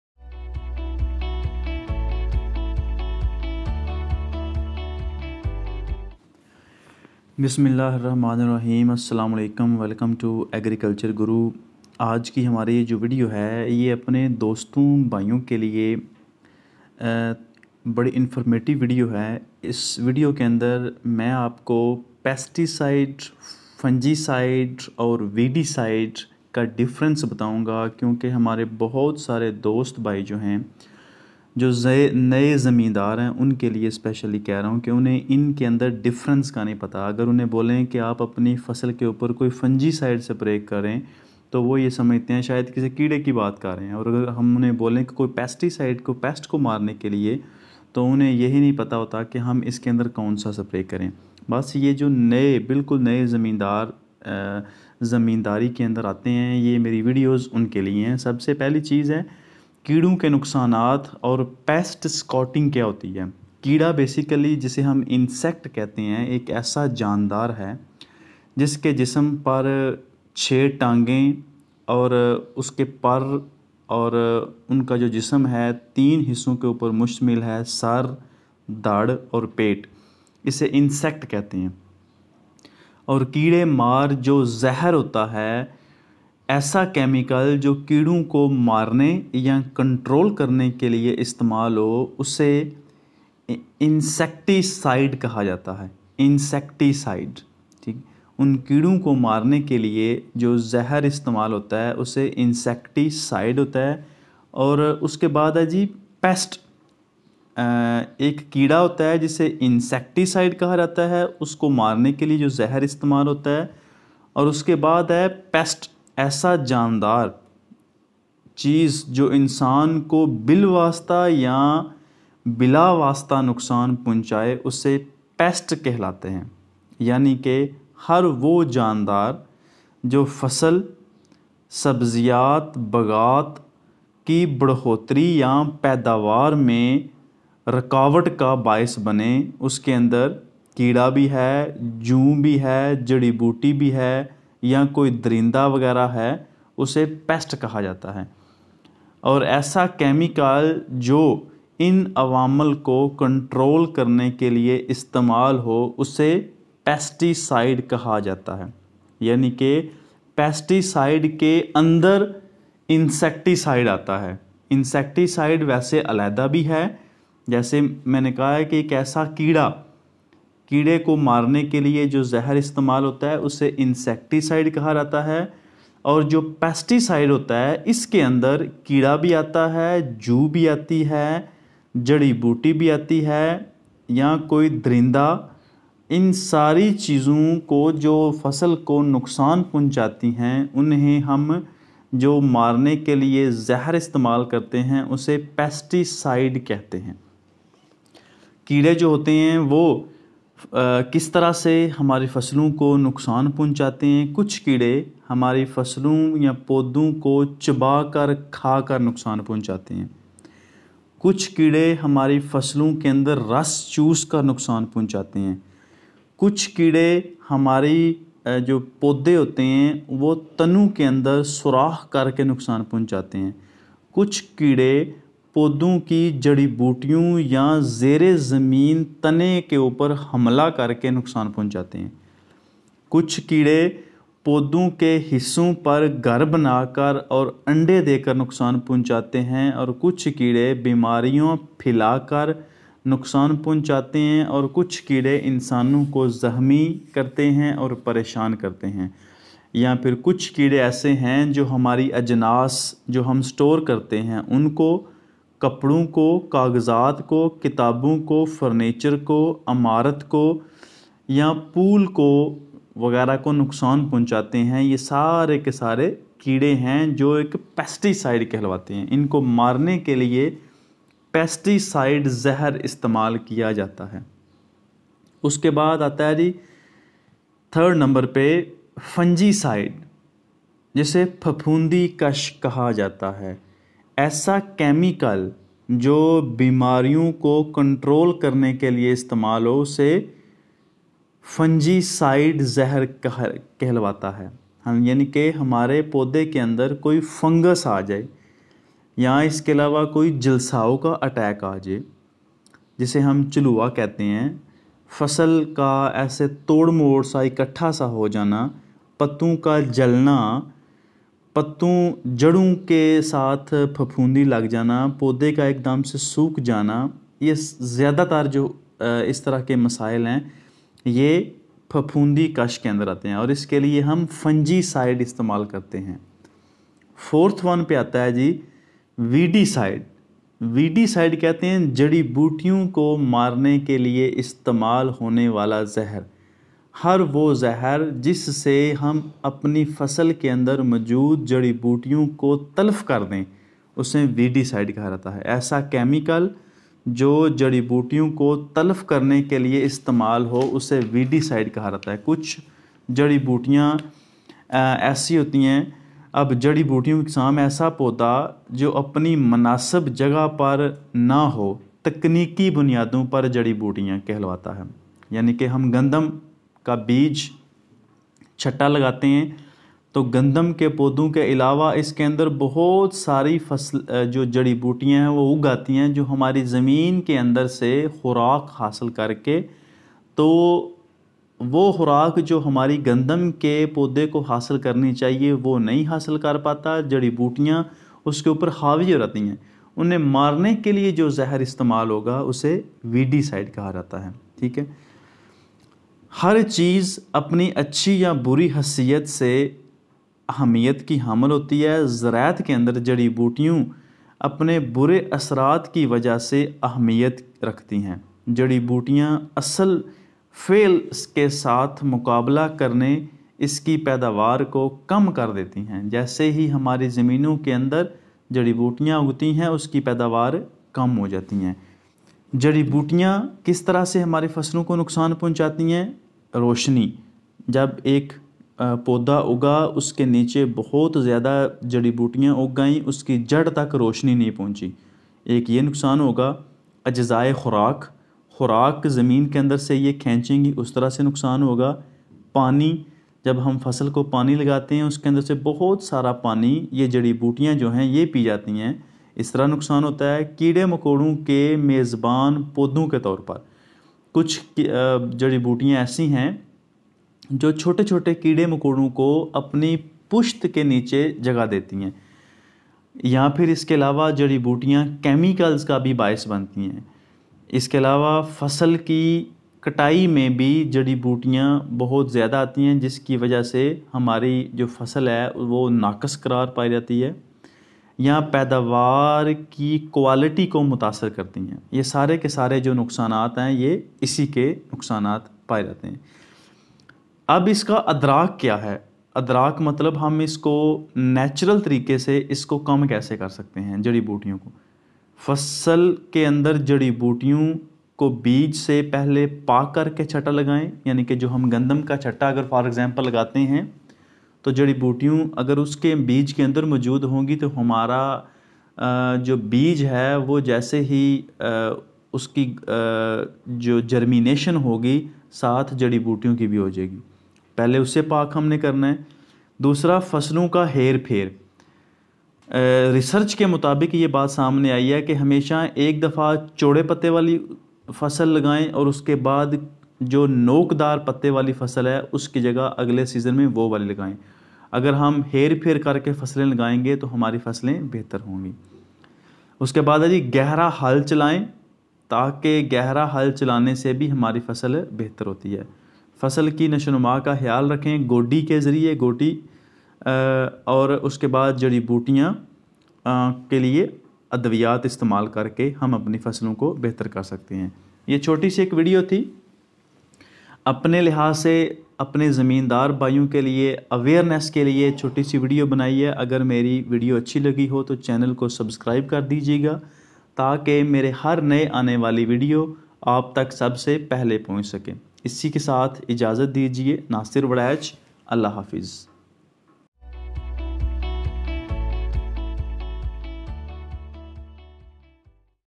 What is pesticide,Insecticide,Fungicide,weedicide?
This is very helpful video for new forms .It's Awesome Video for New Farmers About Pesticide Fungicide  Knowledge .